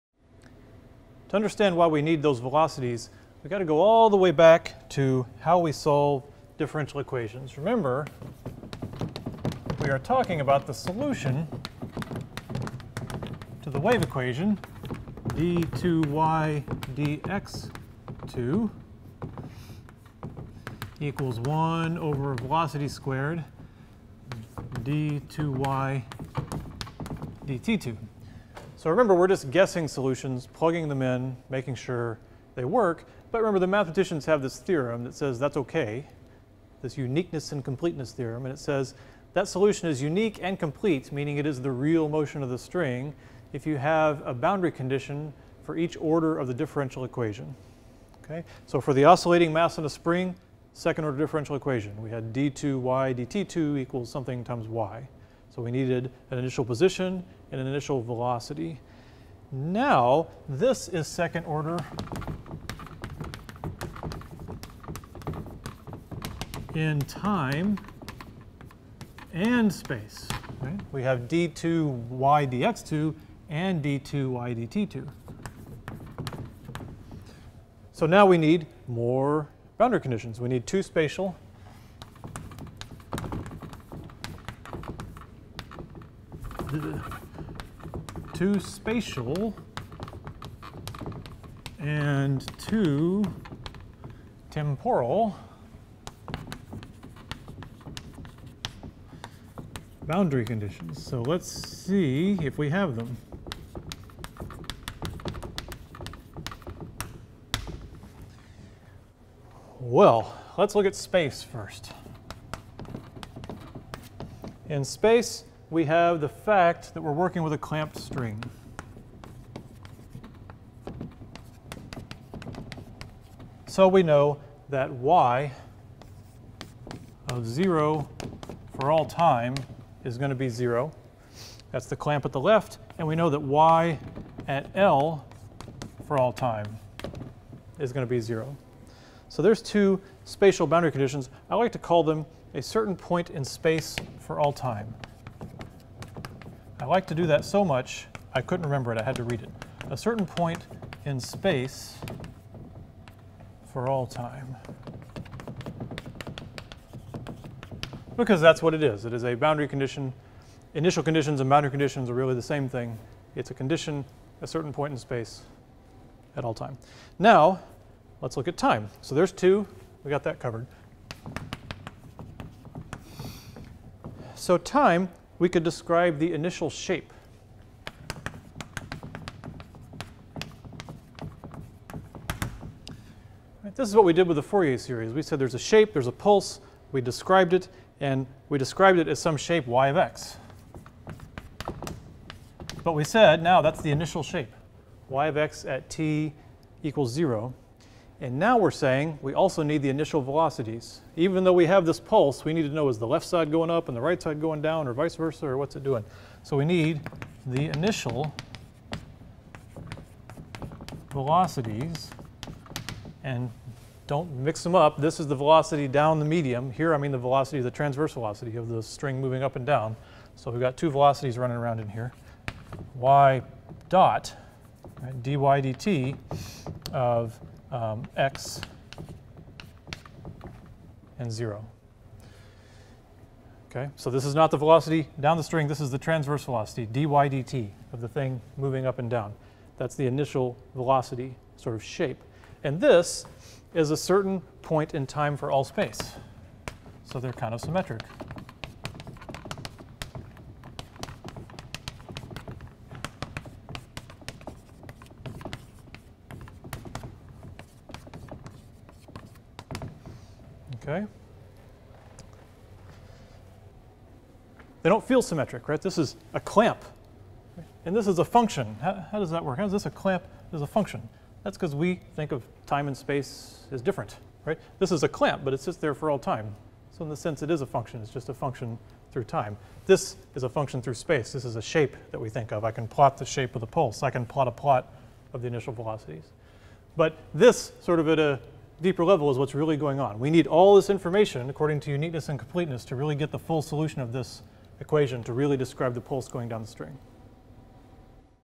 PHYS 201 | Traveling Wave 6 - Boundary Conditions
Here are all of the boundary conditions we will need to fully describe the motion of the string.